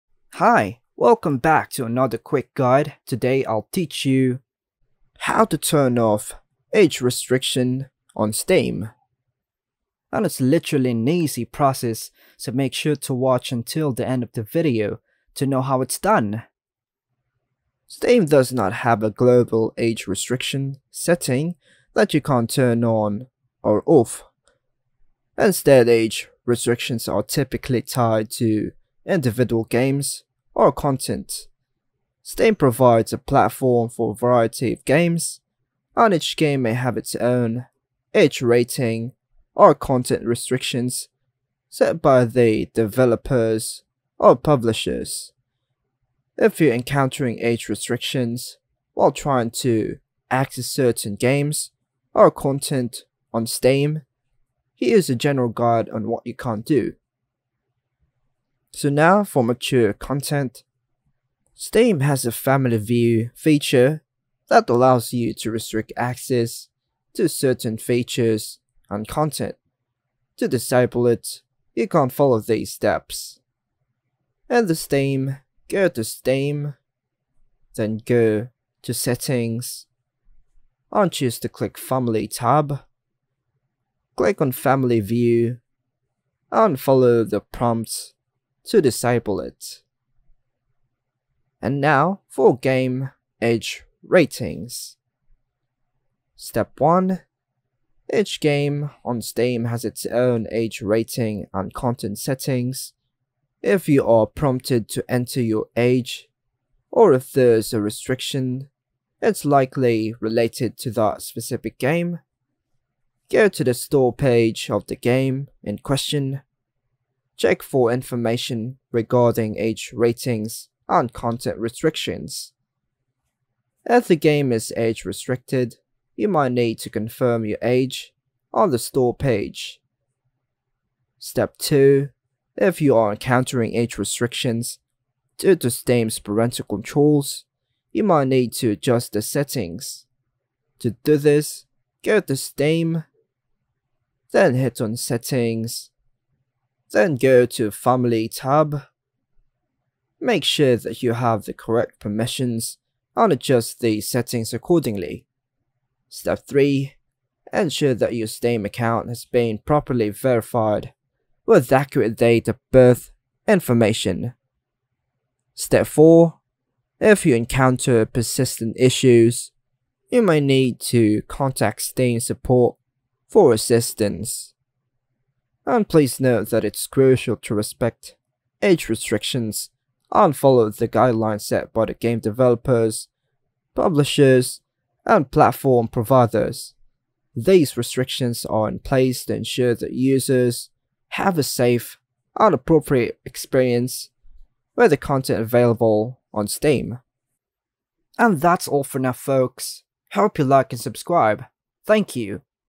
How To Turn Off Age Restriction On Steam (Quick & Easy)
This guide will show you how to turn off age restriction on Steam. Be aware that this will affect the games you see on Steam based on the age restrictions set.